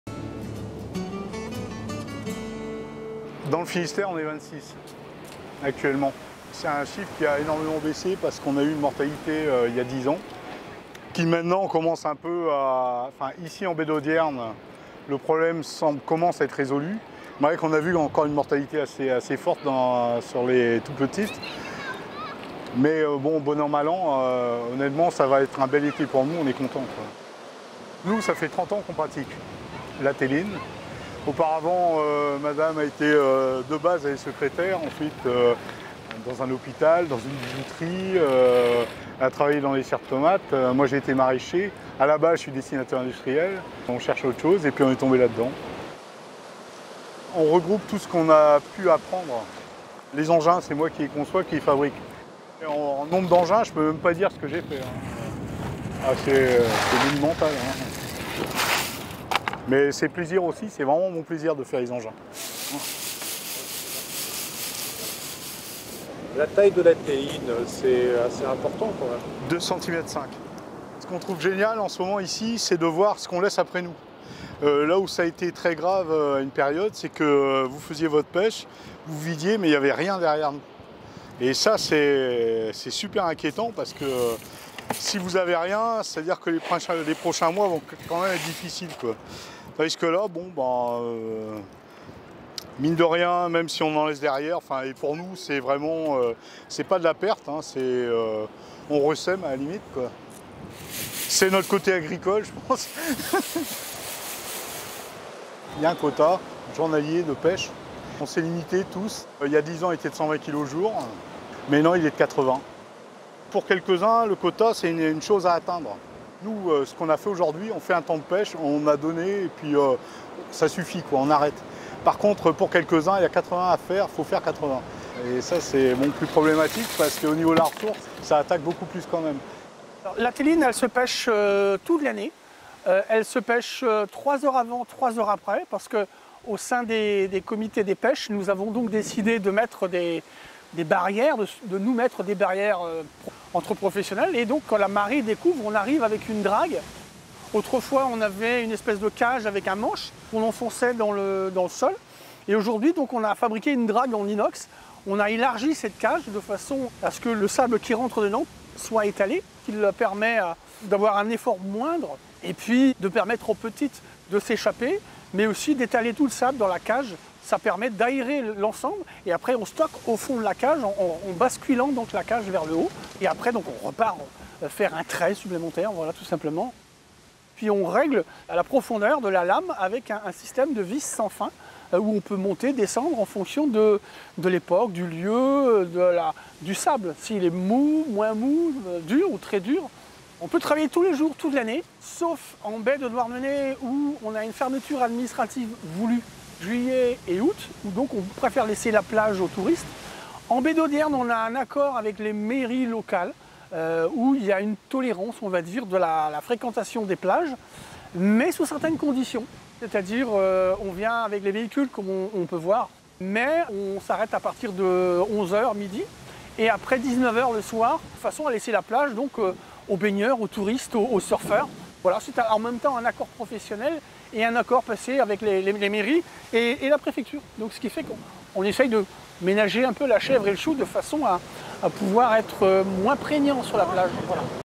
Vidéo pêche de la telline en baie d'Audierne
La plage de Tréguennec à côté de la Torche est également un lieu privilégié pour les surfeurs. Arthur nous la fait découvrir avant de retrouver cet après midi Ronan, pêcheur de tellines.  Il constate que la côte a encore bien diminuée cet hiver,  avec les précédentes tempêtes, et que ces blockhaus, auparavant sur le rivage, se font aujourd’hui recouvrir de vagues.

La telline se développe uniquement dans les zones où il y a un apport d’eau douce. Ce qui est le cas en Baie d’Audierne avec l’exutoire de l’Etang de Trunvel. Mais pendant quelques mois, les pêcheurs n’ont pas pu  récolter de tellines à cause d'une algue toxique.
Ronan espère que le Parc naturel marin d’Iroise lancera des études pour comprendre la diminution de la ressource de tellines. Car il en va de l’avenir de toute une profession.
Le pêcheur de Plomeur, près de Pont-l’Abbé, a débuté en 1987 avec sa femme Jacqueline. « Nous étions alors 250 dans le Finistère. La pression était sans doute trop importante », consent-il. Par la suite, plusieurs crises ont touché la telline. La profession s’est donc organisée pour préserver les populations.
Malgré toutes ces mesures, le nombre de professionnels n’a cessé de baisser.. Ceux qui sont proches de la retraite arrêtent. La moyenne d’âge atteint désormais 50 ans. On est un métier sinistré » Le tonnage a suivi la même courbe descendante : 600 tonnes pêchées dans le Finistère entre 2007 et 2009 ; 70 tonnes l’année dernière. « Pour ramasser le même volume qu’avant, il faut tirer deux à trois fois plus longtemps » Malgré tout, le Bigouden ne veut pas abandonner son métier.. Il s’est passionné pour cette activité qui lui procure une grande liberté et un cadre de travail fabuleux. Enfilant une combinaison de plongée il s’avance dans l’eau, au niveau des premières vagues. Il tire au harnais son chalut-râteau, un gros tamis monté sur roues et dont la lame racle le sable qui est ensuite lessivé par les vagues. Seuls les plus gros galets et les coquillages restent prisonniers. Une fois le chariot plein, il revient sur la plage pour finir de trier les coquillages.
Une fois récoltées, les tellines sont vendues à des mareyeurs comme Tito, qui les expédient en Espagne, Italie, Camargue, ce mollusque bivalve n’étant pas consommé en Bretagne…